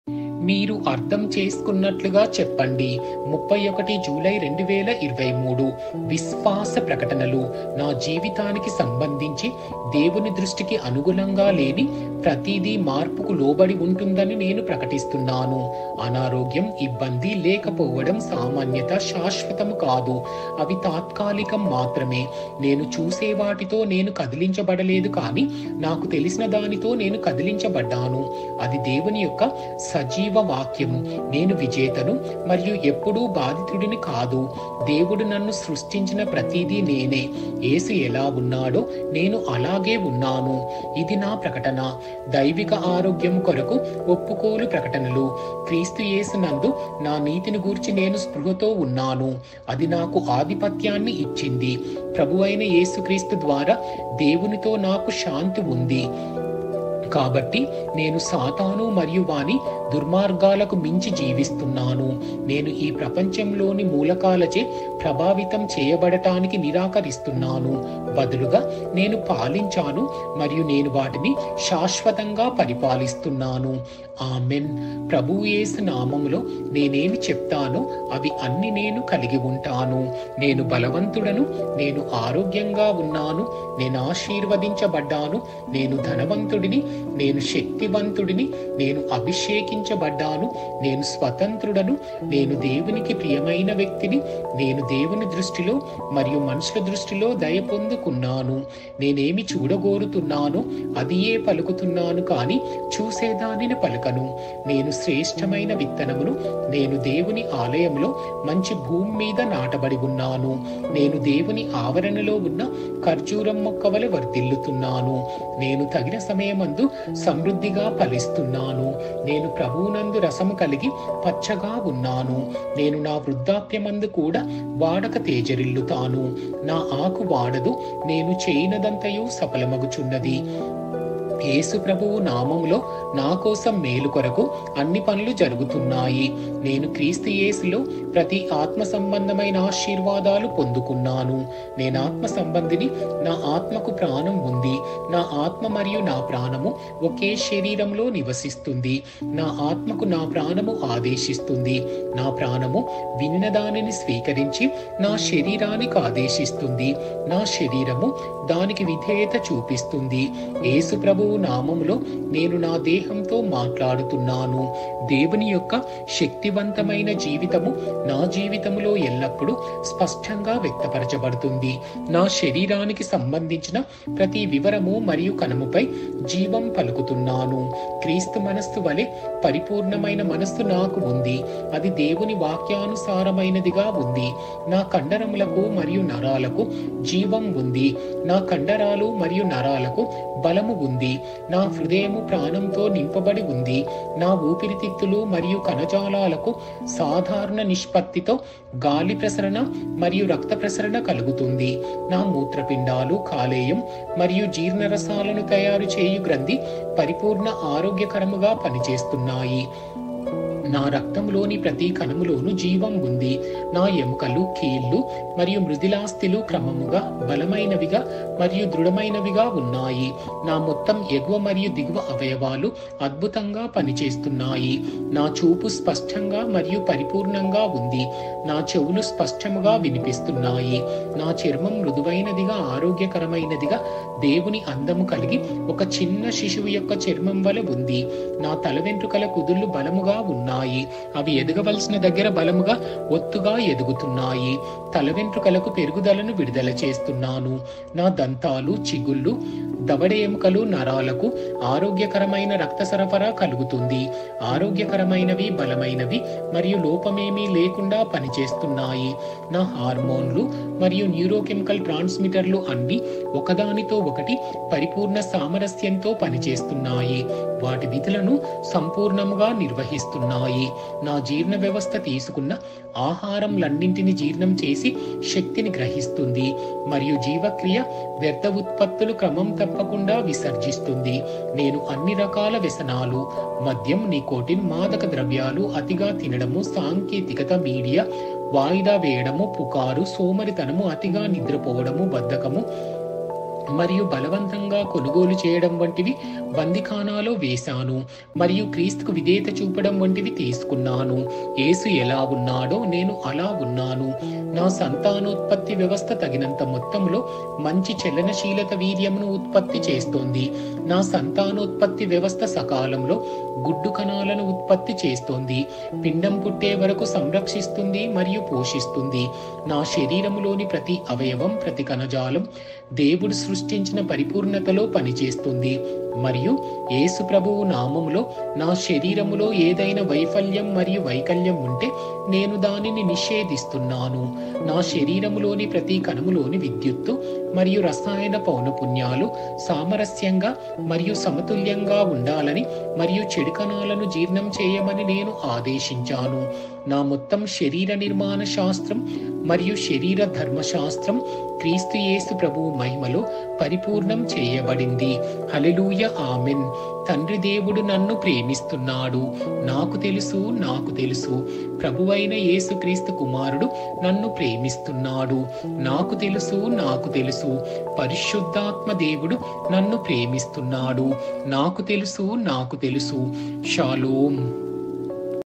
31st June 2023 Say it Like you Mean it(Audio in Telugu)
*మీరు అర్థం చేసుకున్నట్లుగా చెప్పండి*

*31 జూలై 2023*

*విశ్వాస ప్రకటనలు*

నా జీవితానికి సంబంధించి దేవుని దృష్టికి అనుగుణంగా లేని ప్రతిదీ మార్పుకు లోబడి ఉంటుందని నేను ప్రకటిస్తున్నాను. అనారోగ్యం, ఇబ్బంది, లేకపోవడం, సామాన్యత, శాశ్వతం కాదు. అవి తాత్కాలికం మాత్రమే. *నేను చూసే వాటితో నేను కదిలించబడలేదు కానీ నాకు తెలిసిన దానితో నేను కదిలించబడ్డాను, అది "దేవుని యొక్క సజీవ వాక్యం."* నేను విజేతను మరియు ఎప్పుడూ బాధితుడిని కాదు. దేవుడు నన్ను సృష్టించిన ప్రతిదీ నేనే. యేసు ఎలా ఉన్నాడో, నేనూ అలాగే ఉన్నాను. ఇది నా ప్రకటన.

* దైవిక ఆరోగ్యము కొరకు ఒప్పుకోలు ప్రకటనలు*

*క్రీస్తు యేసునందు నా నీతిని గూర్చి నేను స్పృహతో ఉన్నాను, అది నాకు ఆధిపత్యాన్ని ఇచ్చింది.* ప్రభువైన యేసుక్రీస్తు ద్వారా దేవునితో నాకు శాంతి ఉంది కాబట్టి నేను సాతాను మరియు వాని దుర్మార్గాలకు మించి జీవిస్తున్నాను. *నేను ఈ ప్రపంచంలోని మూలకాలచే ప్రభావితం చేయబడటానికి నిరాకరిస్తున్నాను;* బదులుగా, నేను పాలించాను మరియు నేను వాటిని శాశ్వతంగా పరిపాలిస్తున్నాను ఆమెన్!

*ప్రభువు యేసు నామములో నేను ఏమి చెప్తానో అవి అన్ని నేను  కలిగి ఉంటాను (యోహాను 14:13)*

• నేను బలవంతుడను (ఎఫెసీయులకు 6:10)
• నేను ఆరోగ్యంగా ఉన్నాను (1 పేతురు 2:24)
• నేను ఆశీర్వదించబడ్డాను (ఎఫెసీయులకు 1:3)
• నేను ధనవంతుడిని (2 కొరింథీయులు 8:9)
• నేను శక్తివంతుడిని (యెషయా 40:29)
• నేను అభిషేకించబడ్డాను (1 యోహాను 2:20)
• నేను స్వతంత్రుడను (యోహాను 8:36)
• నేను దేవునికి ప్రియమైన వ్యక్తిని (మార్కు 1:11)
• నేను దేవుని దృష్టిలో మరియు మనుషుల దృష్టిలో దయ పొందుకున్నాను (ఆదికాండము 39:2-3)
• నేను ఏమి చూడగోరుతున్నానో అదియే పలుకుతున్నాను, కానీ చూసేదానిని పలుకను (2కొరింథీయులు 4:18)
• నేను శ్రేష్ట్రమైన విత్తనంను (లూకా 8:15)
• నేను దేవుని ఆలయంలో మంచి భూమిమీద నాటబడి ఉన్నాను (కీర్తన 92:13)
• నేను దేవుని ఆవరణలో ఉన్న ఖర్జూరం మొక్కవలె వర్ధిల్లుతున్నాను (కీర్తన 92:13)
• నేను తగిన సమయమందు సమృద్ధిగా ఫలిస్తున్నాను (కీర్తన 1:3)
• నేను ప్రభువునందు రసం కలిగి పచ్చగా ఉన్నాను (కీర్తన 92:14)
• నేను నా వృదాప్యమందు కూడా వాడక  తేజరిల్లుతాను (కీర్తన s 92:14)
• నా  ఆకు వాడదు నేను చేయునదంతయు సఫలమగుచున్నది. (కీర్తన 1:3)
• యేసు ప్రభువు నామంలో నా కోసం మేలు కొరకు అన్ని పనులు జరుగుతున్నాయి. (రోమీయులకు 8:28)
• నేను క్రీస్తు యేసులో ప్రతి ఆత్మ సంబంధమైన ఆశీర్వాదాలు  పొందుకున్నాను. (ఎఫెసీయులకు 1:3)

• నేను ఆత్మ సంబంధిని
• నా ఆత్మకు ప్రాణము ఉంది
• నా ఆత్మ మరియు నా ప్రాణము ఓకే శరీరంలో నివసిస్తుంది
• నా ఆత్మకు నా ప్రాణము ఆదేశిస్తుంది.  నా ప్రాణము వినినదానిని స్వీకరించి నా శరీరానికి ఆదేశిస్తుంది, నా శరీరము దానికి విధేయత చూపిస్తుంది .

*యేసు ప్రభువు నామంలో, నేను నా దేహంతో మాట్లాడుతున్నాను*

• దేవుని యొక్క శక్తి వంతమైన జీవితము, నా జీవితంలో ఎల్లప్పుడూ స్పష్టంగా వ్యక్తపరచబడుతుంది .
• నా శరీరానికి సంబంధించిన ప్రతి వివరము/ కణము పై జీవము పలుకుతున్నాను
✝️క్రీస్తు మనస్సు వలె పరిపూర్ణమైన  మనస్సు  నాకు ఉంది, అది దేవుని వాక్యానుసారమైనదిగా ఉంది.
✝️నా కండరమూలకు మరియు  నరాలకు జీవము ఉంది
✝️నా కండరాలు మరియు నరాలకు బలము ఉంది
✝️నా హృదయము ప్రాణముతో నింపబడి ఉంది
✝️నా ఊపిరితిత్తులు మరియు కణజాలాలకు సాధారణ నిష్పత్తితో గాలి/వాయు ప్రసరణ మరియు రక్త ప్రసరణ కలుగుతుంది.
✝️నా మూత్రపిండాలు, కాలేయం మరియు జీర్ణరసాలను తయారుచేయు గ్రంథి పరిపూర్ణ  ఆరోగ్యకరముగా  పని చేస్తున్నాయి
✝️నా రక్తంలోని ప్రతి కణంలోనూ జీవము ఉంది
✝️నా ఎముకలు, కీళ్ళు మరియు మృదులాస్థిలు క్రమముగా, బలమైనవిగ మరియు దృఢమైనవిగ ఉన్నాయి
✝️నా మొత్తం ఎగువ మరియు దిగువ అవయవాలు అద్భుతంగా పని చేస్తున్నాయి
✝️నా చూపు స్పష్టంగా మరియు పరిపూర్ణంగా ఉంది
✝️నా చెవులు స్పష్టముగా వినిపిస్తున్నాయి
✝️నా చర్మం మృదువైనదిగ, ఆరోగ్యకరమైనదిగా, దేవుని అందం కలిగి ఒక చిన్న శిశువు యొక్క చర్మం వలె ఉంది
✝️నా తల వెంట్రుకల కుదుళ్లు బలముగా ఉన్నాయి,  అవి ఎదగవలసిన దగ్గర బలముగా ఒత్తుగా ఎదుతున్నాయి, తల వెంట్రుకలకు పెరుగుదలను విడుదల చేస్తున్నాను
✝️నా దంతాలు, చిగుళ్ళు, దవడ ఎముకలు, నరాలకు ఆరోగ్యకరమైన రక్త సరఫరా కలుగుతుంది,  ఆరోగ్యకరమైనవి, బలమైనవి మరియు లోపం ఏమి లేకుండా పని చేస్తున్నాయి.
✝️నా హార్మోన్లు మరియు న్యూరోకెమికల్ ట్రాన్స్‌మిటర్లు అన్నీ ఒకదానితో ఒకటి పరిపూర్ణ సామరస్యంతో పని చేస్తున్నాయి, వాటి విధులను సంపూర్ణముగా నిర్వహిస్తున్నాయి
✝️నా జీర్ణవ్యవస్థ తీసుకున్న ఆహారములన్నింటినీ జీర్ణం చేసి, శక్తి ని గ్రహిస్తుంది మరియు జీవక్రియ వ్యర్థ ఉత్పత్తులుక్రమం తప్పకుండా విసర్జిస్తుంది
✝️నేను అన్ని రకాల *వ్యసనాలు (మద్యం, నికోటిన్, మాదకద్రవ్యాలు)*, *అతిగా తినడం, సాంకేతికత, మీడియా, వాయిదా వేయడం, పుకారు, సోమరితనం, అతిగా నిద్రపోవడం, బద్ధకం మరియు బలవంతంగా కొనుగోలు చేయడం వంటివి* బందిఖానాలో వేసాను మరియు క్రీస్తుకు విధేయత చూపడం వంటివి తీసుకున్నాను. యేసు ఎలా ఉన్నాడో నేను అలా ఉన్నాను*
✝️🙎🏻‍♂️ ♂️ నా సంతానోత్పత్తి వ్యవస్థ తగినంత మొత్తంలో మంచి చలనశీలత వీర్యమును ఉత్పత్తి చేస్తుంది
✝️🙍🏻‍♀️ ♀️ నా  సంతానోత్పత్తి వ్యవస్థ సకాలంలో గుడ్డు కణాలను ఉత్పత్తి చేస్తుంది, పిండం పుట్టే వరకు సంరక్షిస్తుంది మరియు పోషిస్తుంది *
✝️ నా శరీరంలోని ప్రతి అవయవం, ప్రతి కణజాలం దేవుడు సృష్టించిన పరిపూర్ణతలో పని చేస్తుంది మరియు యేసుప్రభువు నామంలో నా శరీరంలో ఏదైనా వైఫల్యం మరియు వైకల్యం ఉంటె నేను దానిని  నిషేధిస్తున్నాను.